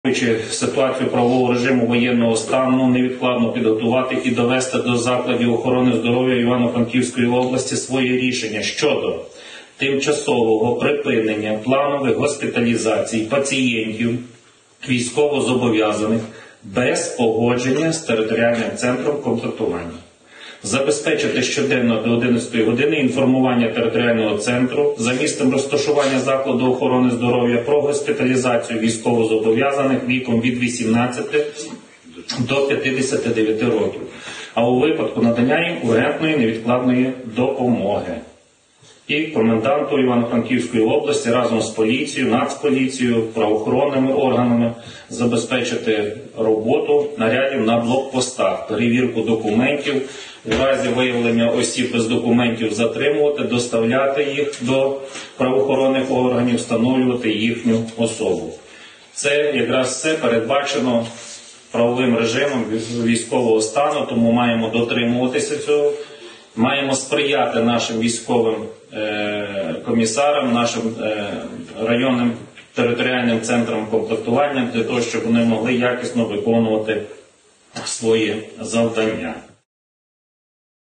Військовозобов‘язаних не госпіталізуватимуть в лікарнях Івано-Франківщини без дозволу військкоматів
Згідно з рішенням ради оборони Івано-Франківської  області, чоловіки повинні отримати дозвіл ТЦК на госпіталізацію. Також посилюється робота на блокпостах: усіх, хто не має з собою документів, відправлятимуть у відділок для встановлення особи.